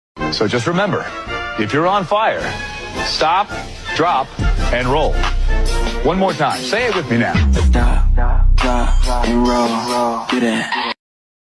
//Stop, Drop and Roll// OC // GL2
yup, we’re back with these ridiculous, silly skits💀
and NO this wasn’t made out of boredom, it was made out of fun :D

⋆°⋄⊹☆Characters Used☆⋆°⋄⊹
Hope *(My OC)*
⋆°⋄⊹☆Apps Used☆⋆°⋄⊹
Gacha Life 2
Alright Motion
Capcut